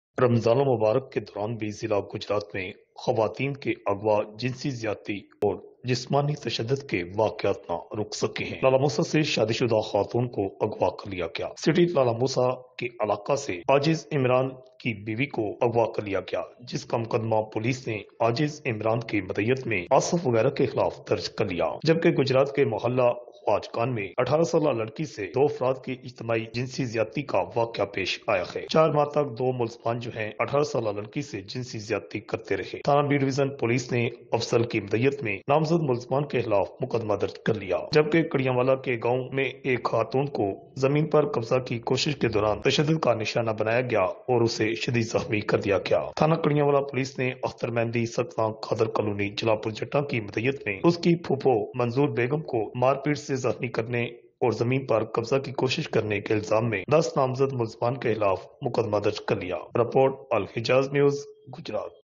A woman married to Lala Musa was abducted. A young girl was gang-raped in Gujarat. See what happened
لالہ موسیٰ سے شادی شدہ خاتون اغواء گجرات میں جوانسالہ لڑکی سے اجتماعی زیادتی کڑیانوالہ کے گاؤں ڈوڈھے میں خاتون پر مسلح افراد کا تشدد ملزمان۔ کا کیا بنا دیکھئے الحجاز نیوز کی اس رپورٹ میں